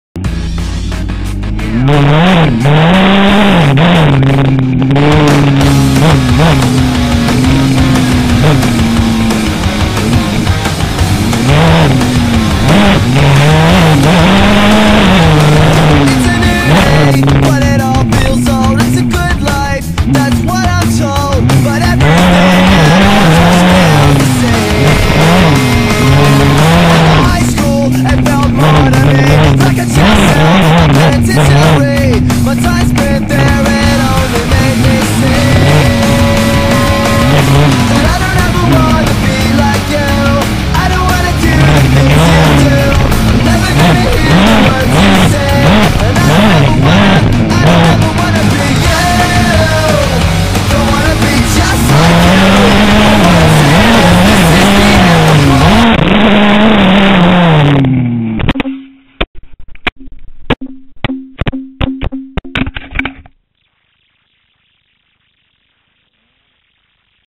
Go Pro Helmet Cam Falls off while riding
Just riding around and the camera mount completely comes off. Mounted on the front number plate. Took 20 minutes to find the camera. The background noise is the engine. The reason why the engine sounds so wierd is just because of where I mounted it. Braaap!